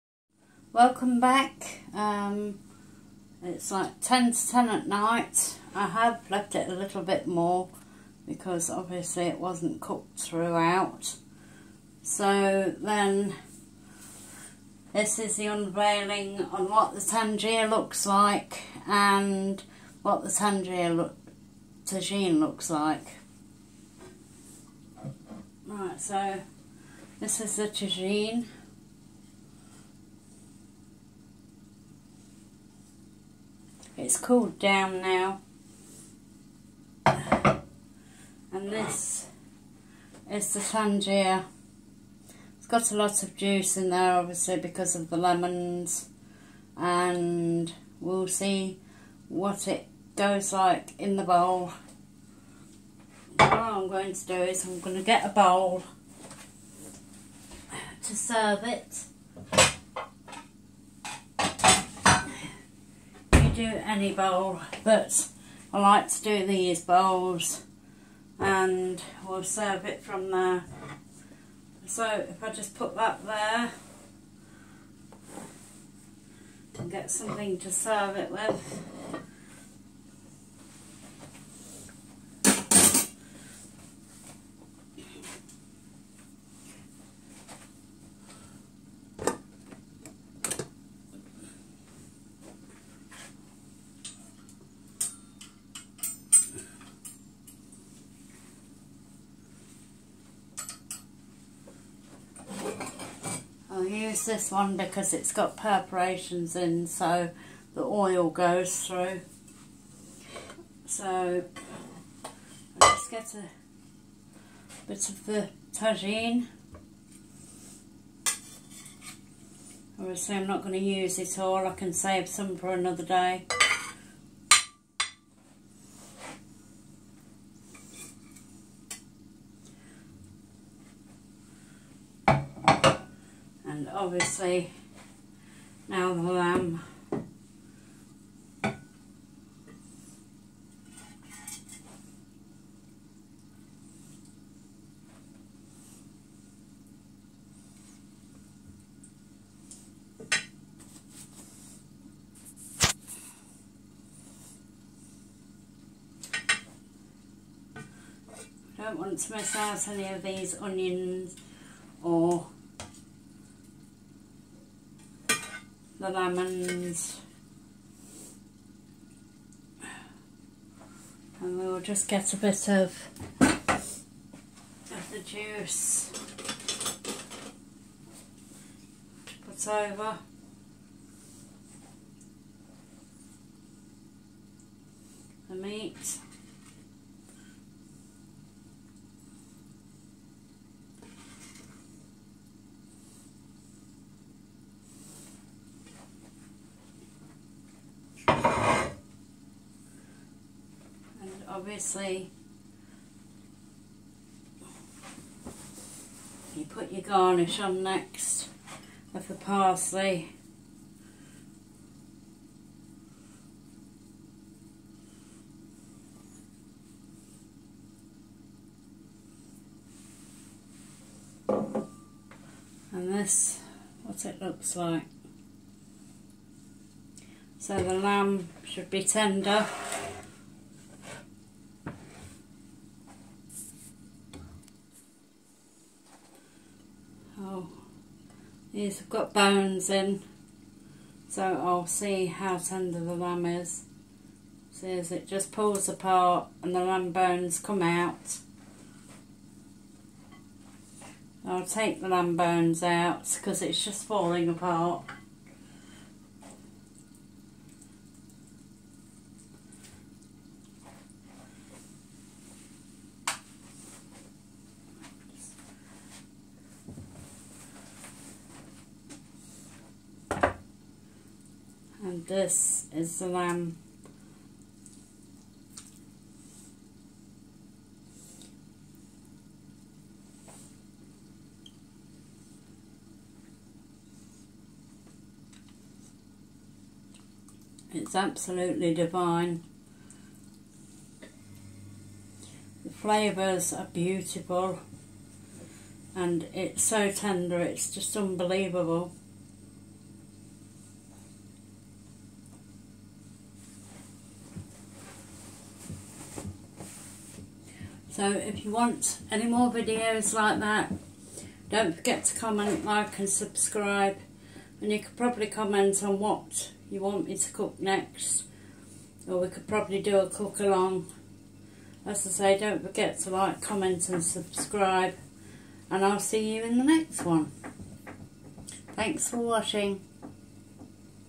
moroccan tanjia with vegetable tagine
finale to this epic and  famous marrakech dish melt in the mouth moroccan lamb tanjia with a vegetable tagine served together.